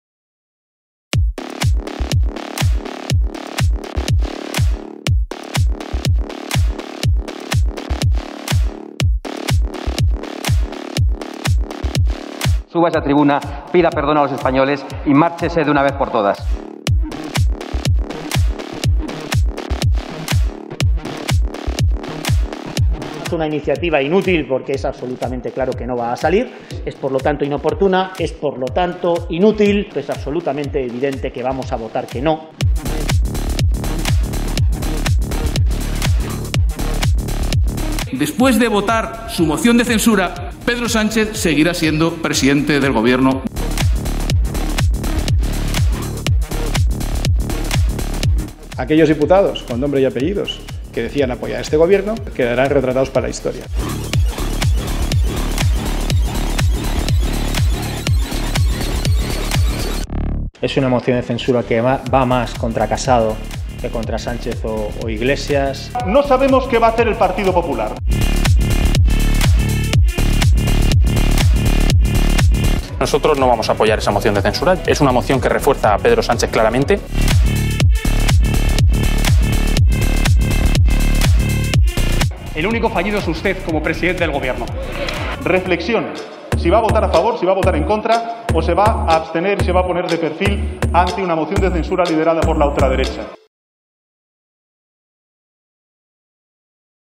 Una moción a favor de aumentar la tensión política | Comunicación Política
Todas las miradas se concentran en el PP que tiene que decidir si se aparta de la ultraderecha o la secunda.